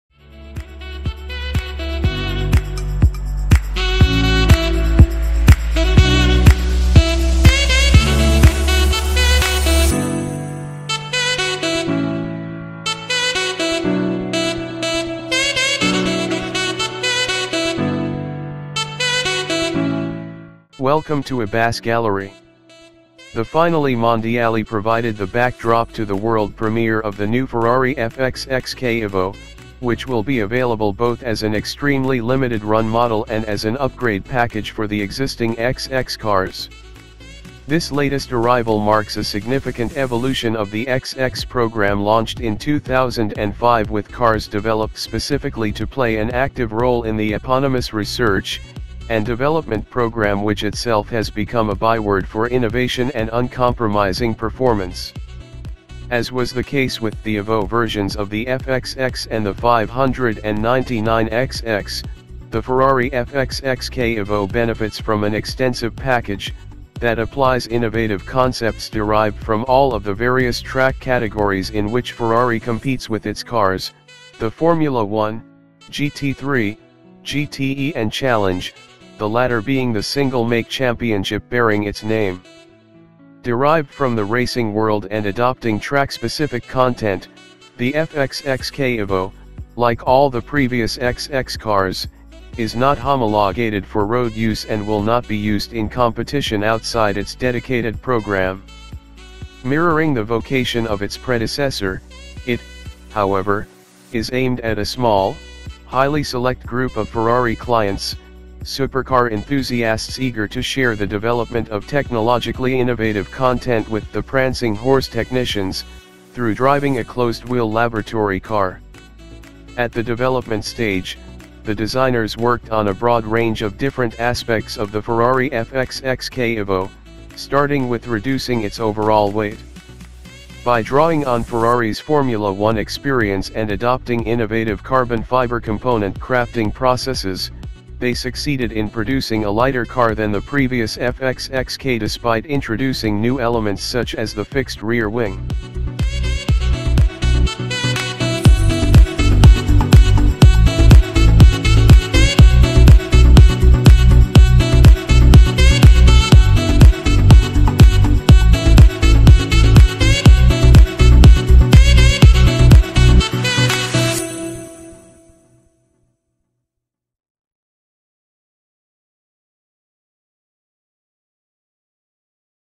2018 Ferrari FXX-K Evo
2018 Ferrari FXX-K Evo, the Finali Mondiali provided the backdrop to the world premiere of the new Ferrari FXX-K Evo which will be available both as an extremely limited-run model and as an upgrade package for the existing XX cars. This latest arrival marks a significant evolution of the XX programme launched in 2005 with cars developed specifically to play an active role in the eponymous research and development programme which itself has become a byword for innovation and uncompromising performance. As was the case with the Evo versions of the FXX and the 599 XX, the Ferrari FXX-K Evo benefits from an extensive package that applies innovative concepts derived from all of the various track categories in which Ferrari competes with its cars: the Formula 1, GT3, GTE and Challenge, the latter being the single-make championship bearing its name.